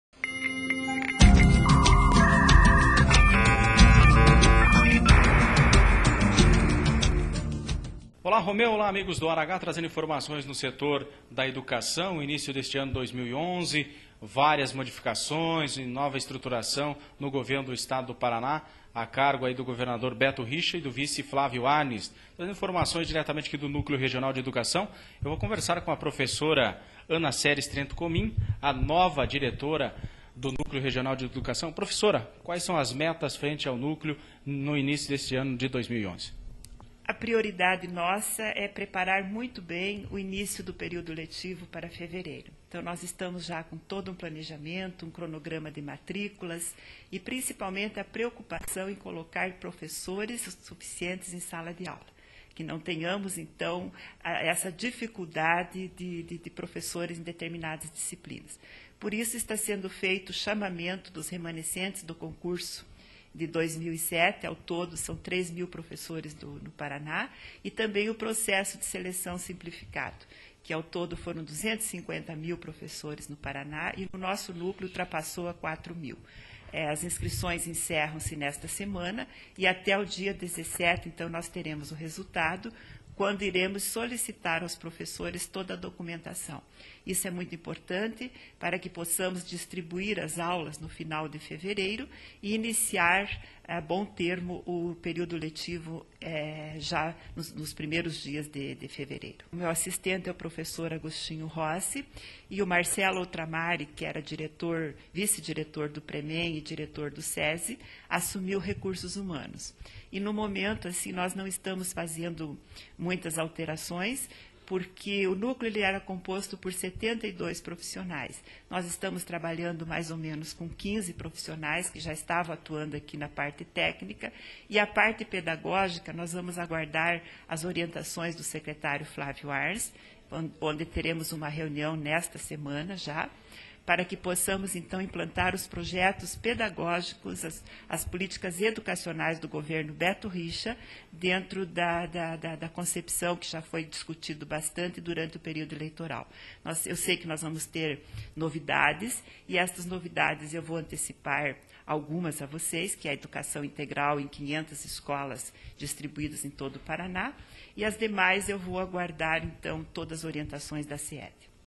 EDUCAÇÃO Ana Seres Trento Comin, fala sobre os trabalhos frente ao núcleo regional.wmv
Assumiu recentemente o cargo de chefe do Núcleo Regional de Educação, em Pato Branco, Ana Seres Trento Comin. O repórter Luciano Bernardi, conversou com a professora que fala sobre os principais projetos frente ao núcleo de educação.